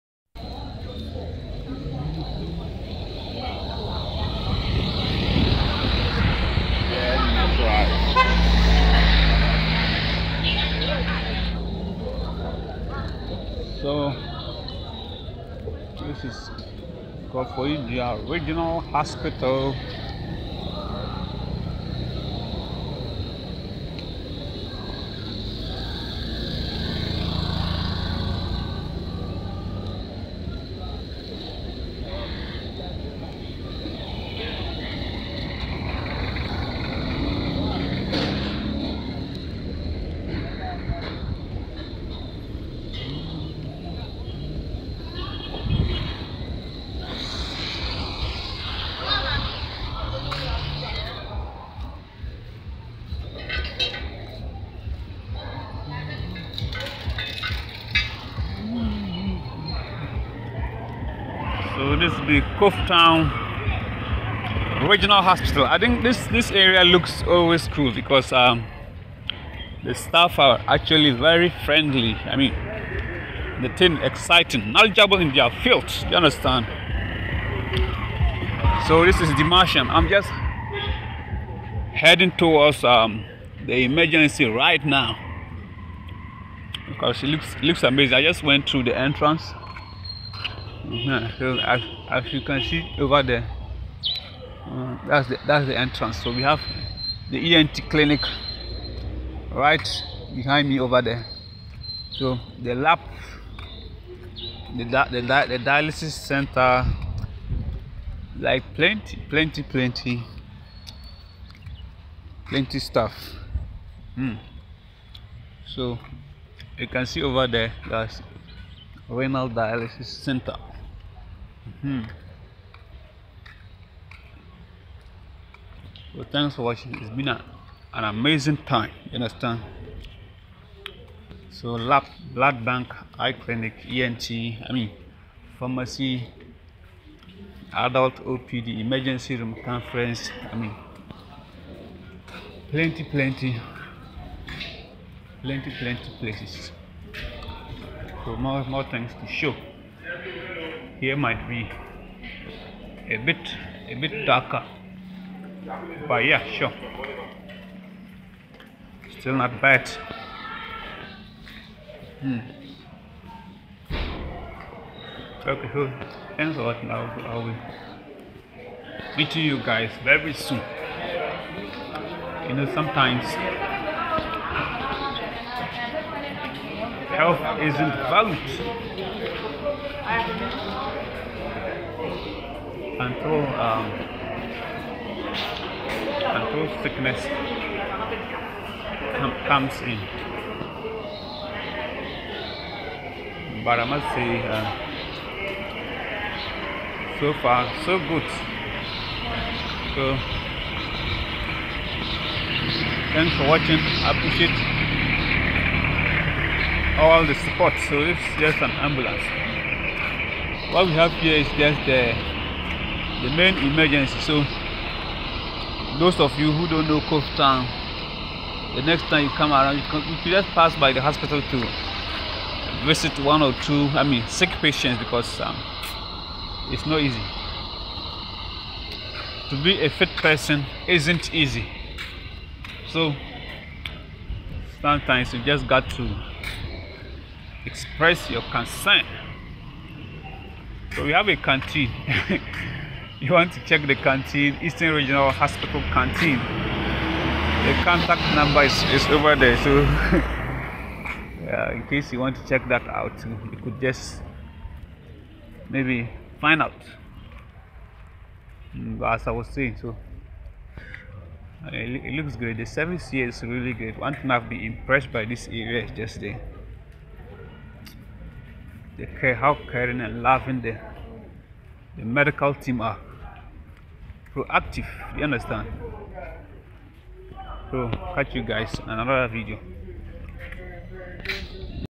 Koforidua Regional Hospital Live
Demarshian takes you on a tour of Koforidua Regional Hospital Live